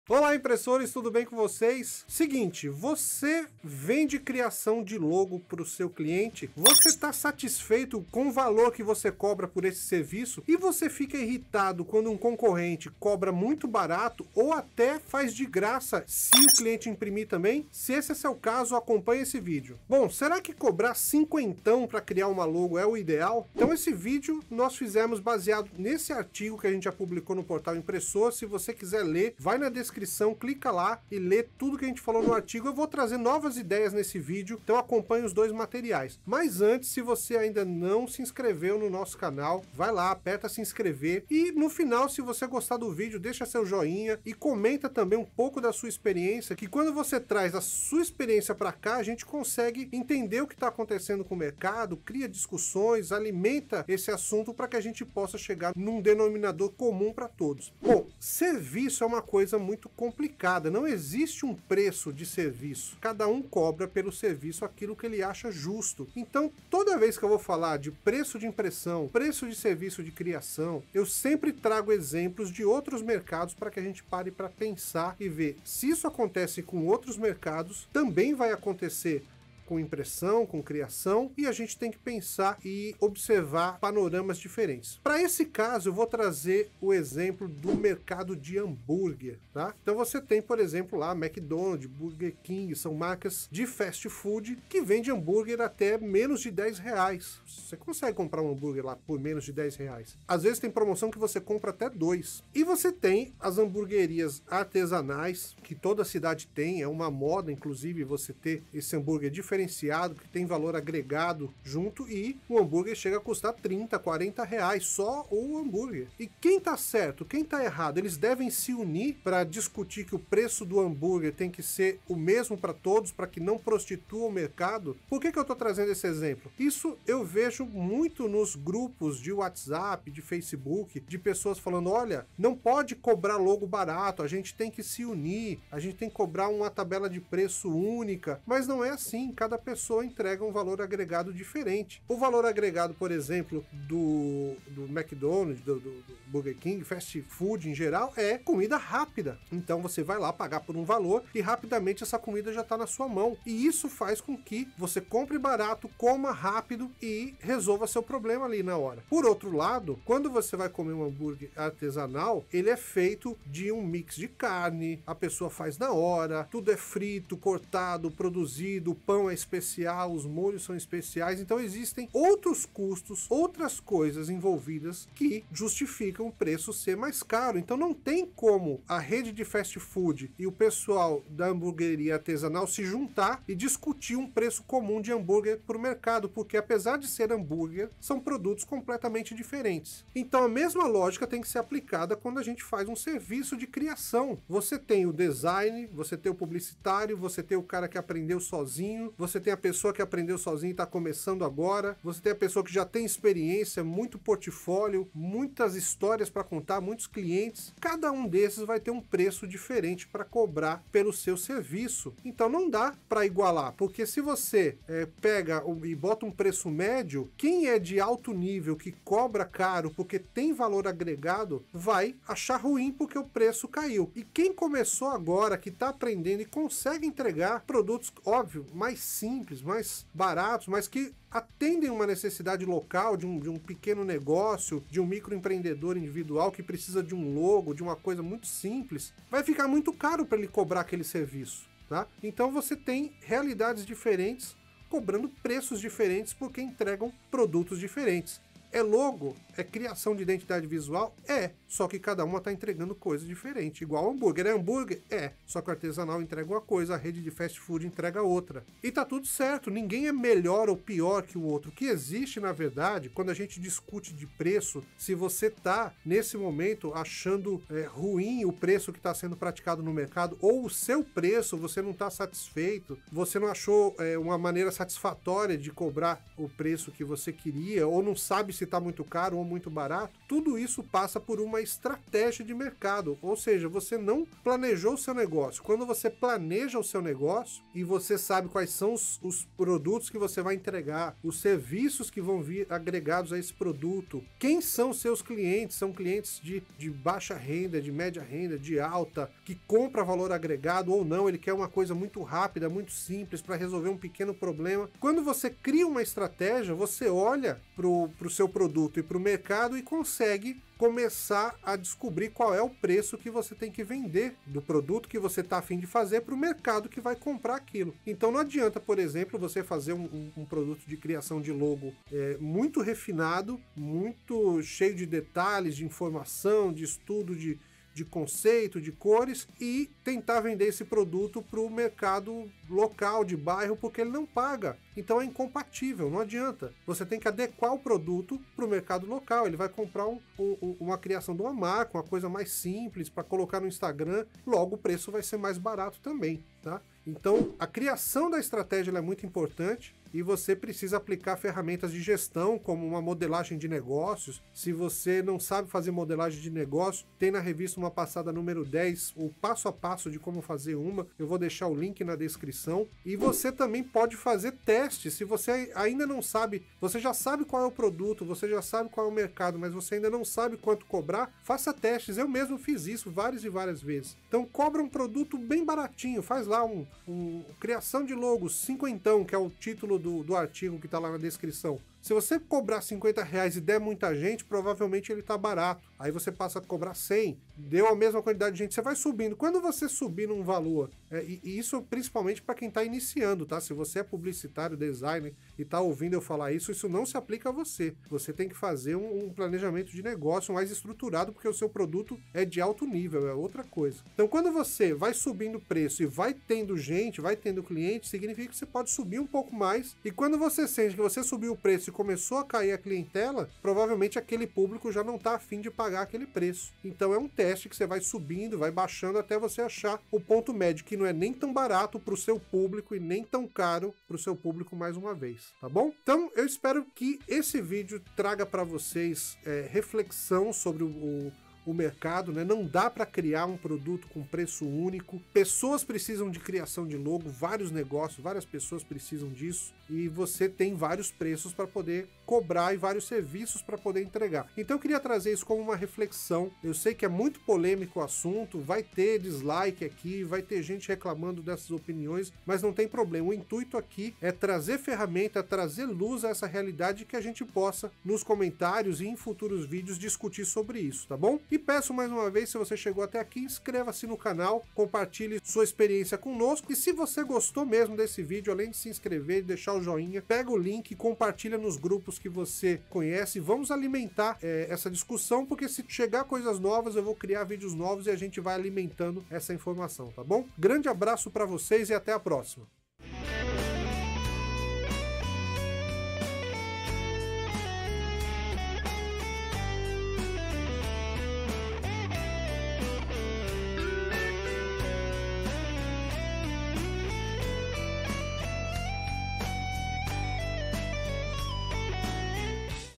Quanto Custa Criar Uma Logo? R$50tão?
Quanto custa criar uma logo é uma dúvida comum, principalmente para quem está começando e se depara com valores de R$50 a R$5.000. Afinal de contas, a logo é um dos principais identificadores visuais da sua marca.

Não tem problema nenhum cobrar R$50ão na logo, desde que haja uma estratégia ou estudo nisso.

Não dá para generalizar se R$50ão é bom ou é ruim. Sempre quando se discute preço, eu uso o seguinte exemplo: quem está certo ou quem está errado vendendo hambúrguer?

Gostou do vídeo? Então NÃO SE ESQUEÇA

Conecte-se com todos nossos canais!